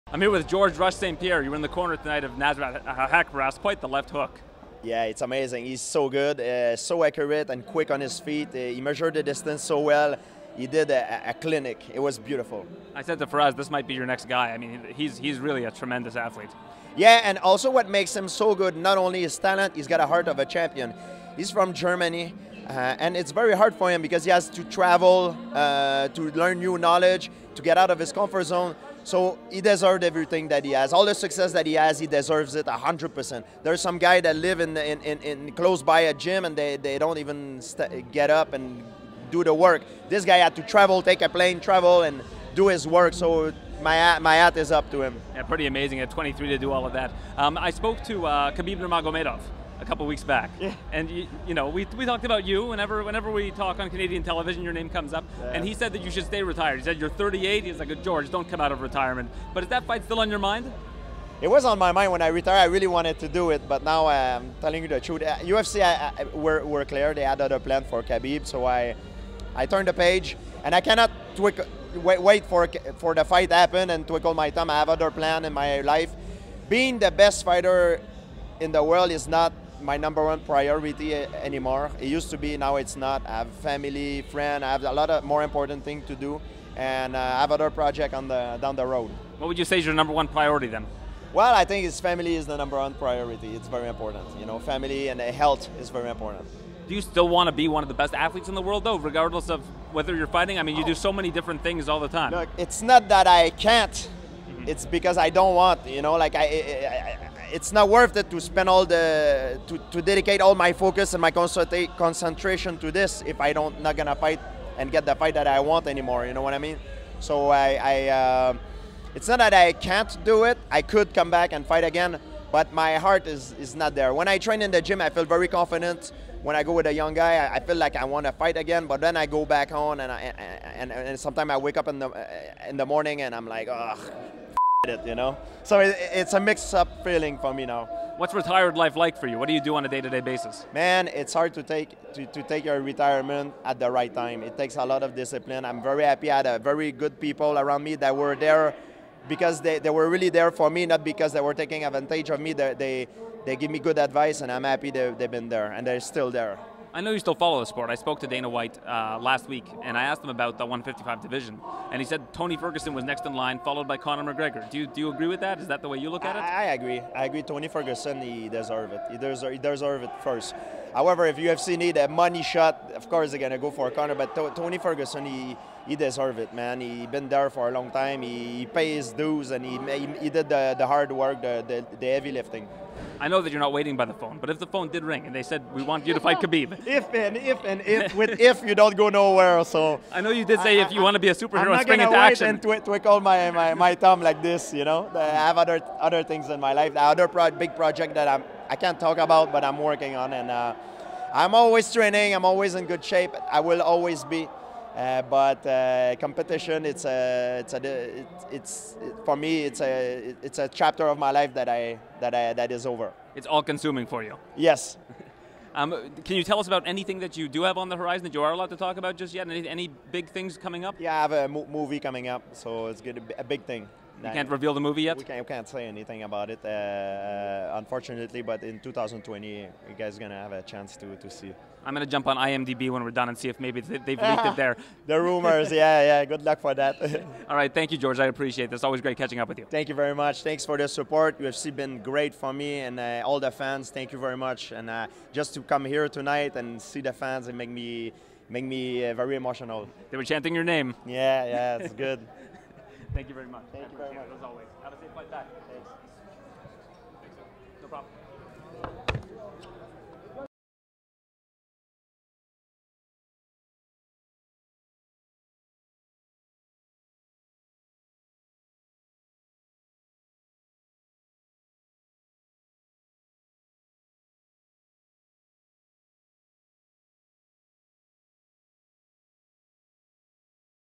GSP: Fighting is not my top priority anymore
Canadian MMA legend Georges St-Pierre dishes on what retirement life has been like for him and whether or not he would consider returning to the UFC.